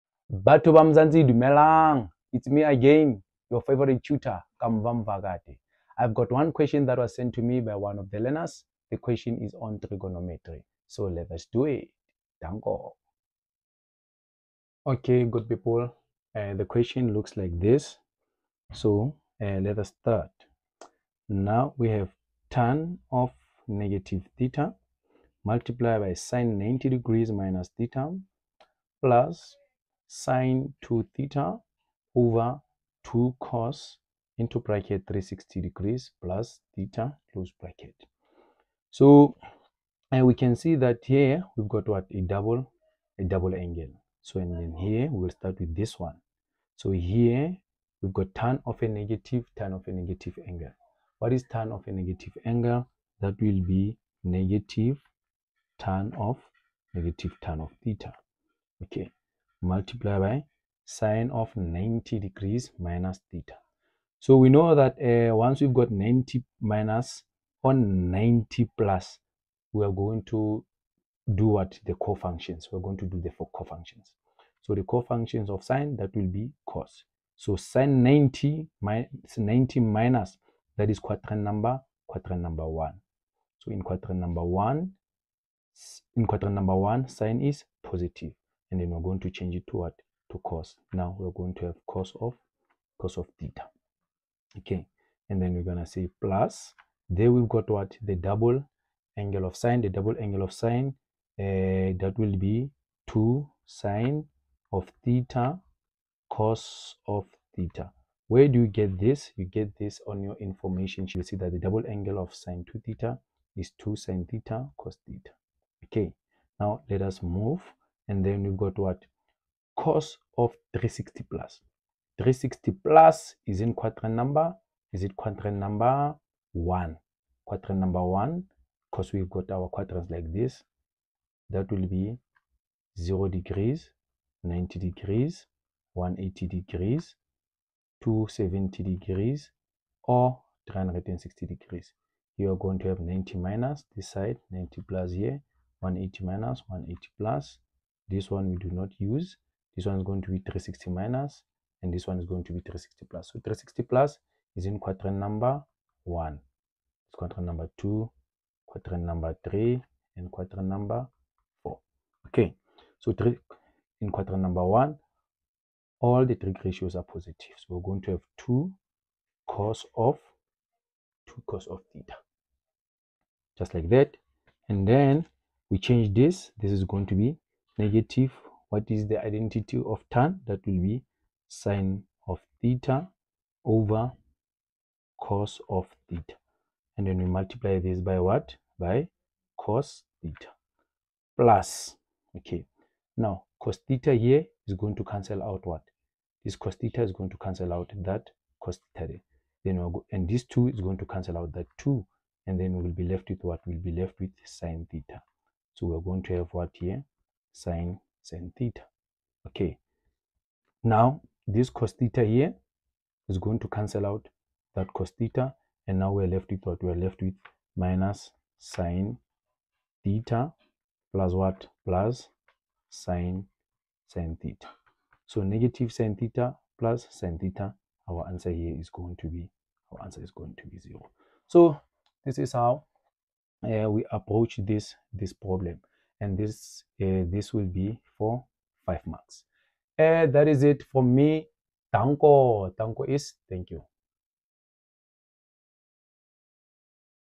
Trigonometry |Grade 12| Problem Solving
Here I’m solving a trig problem that was sent to me by one of the learners in grade 12… Mathematics Made simple…